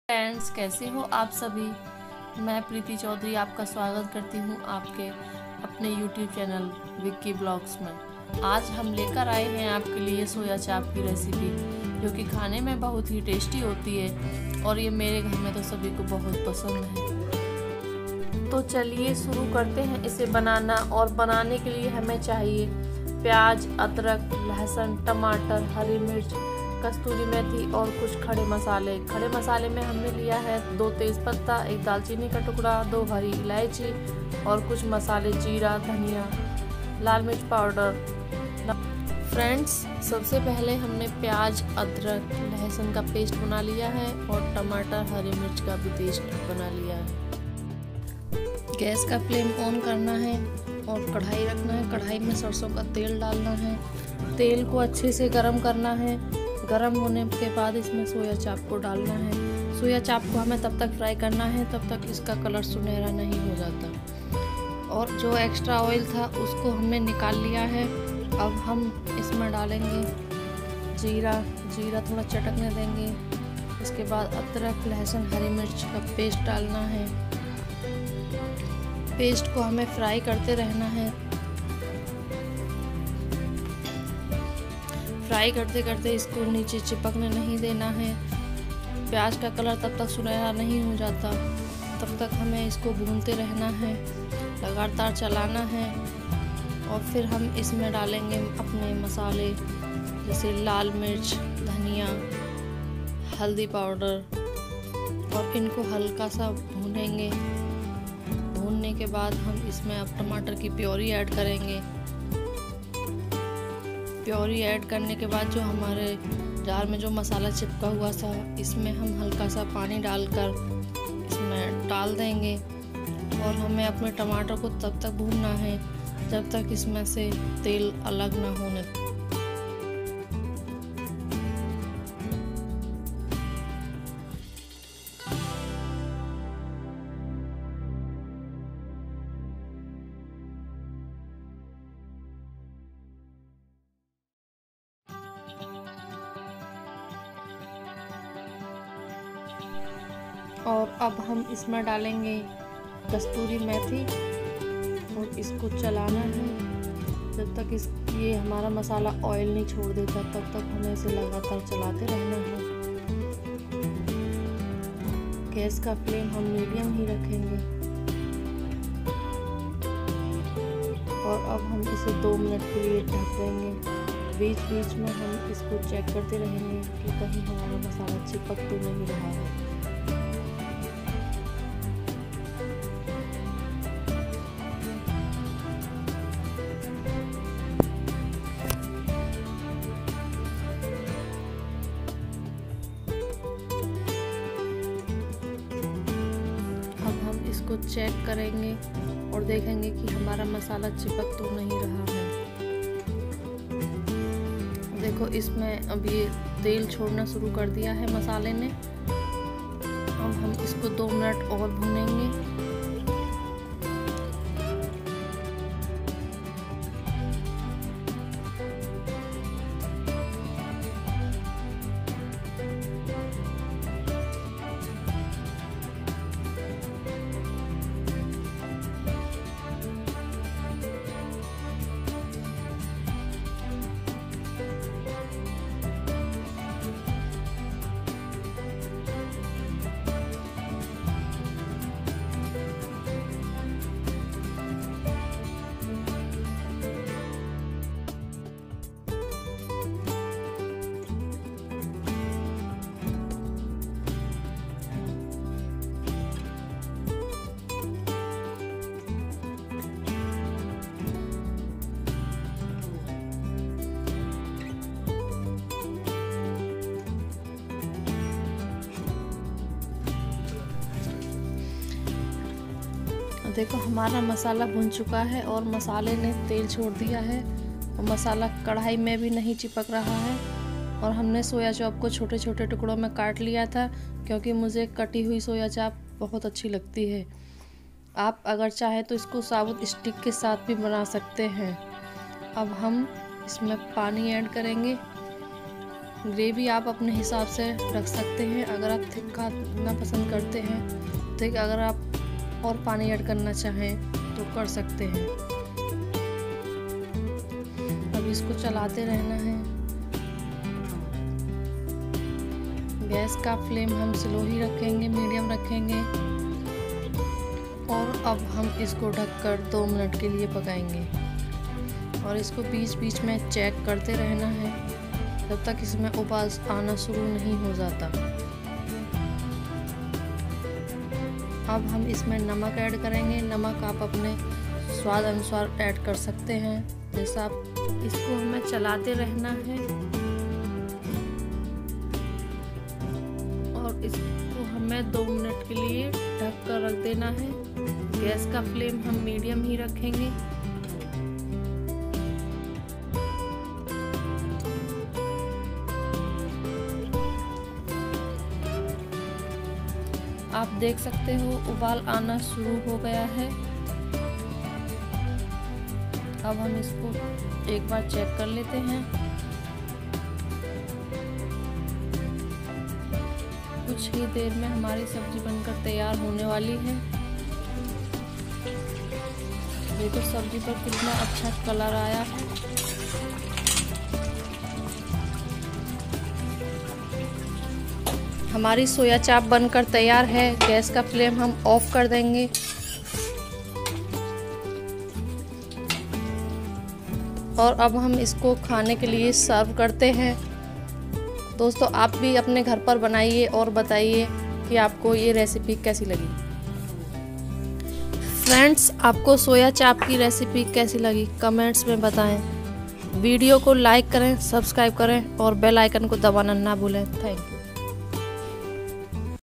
Soya Chaap Recipe : ढाबा Style : इस तरह बनाओगे तो उँगलियाँ चाटने लगोगे क्योकि ये recipe है सब से अलग
Welcome to Vicky Vlogs, Soya मलाई चाप recipe in my style, which is very easy to make with available ingredients at our home.